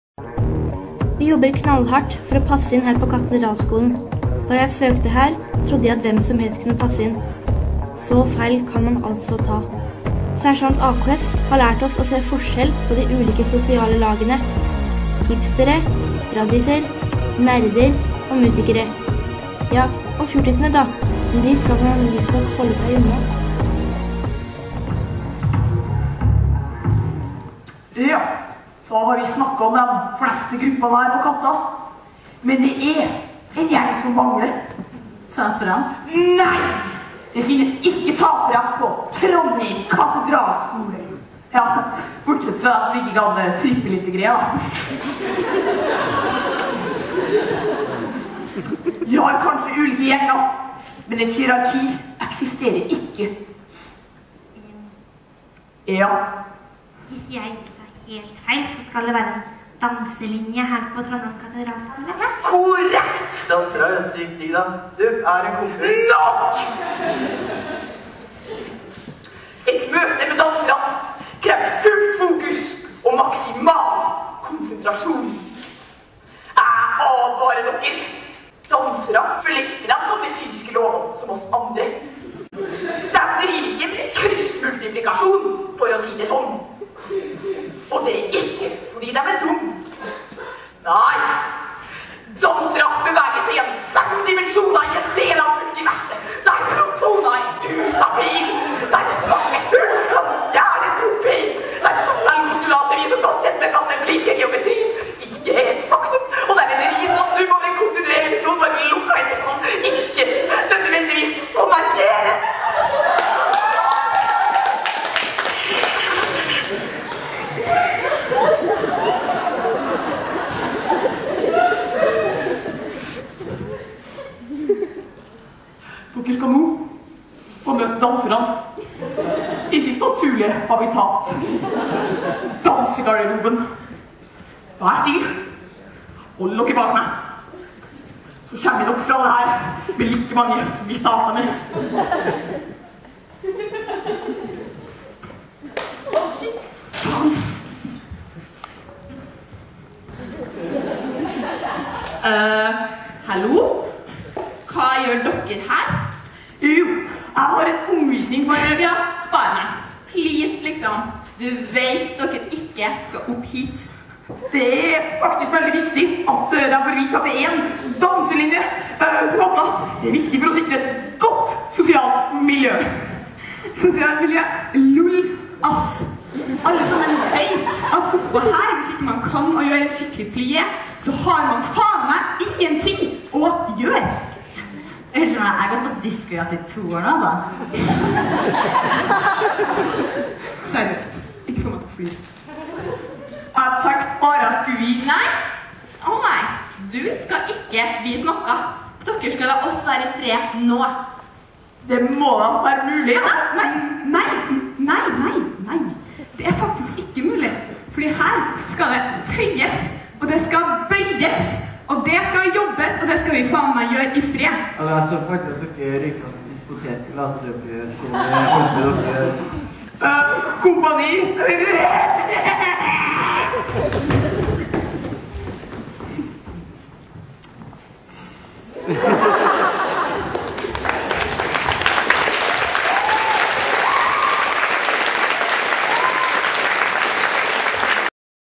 15 AKS 3 - Det finnes en danselinje (Russerevyen på Katta 2012)
Russerevyen ved Trondheim Katedralskole 2012
Julie, Ingunn, Sofia, Johan, Kristin H og Mari H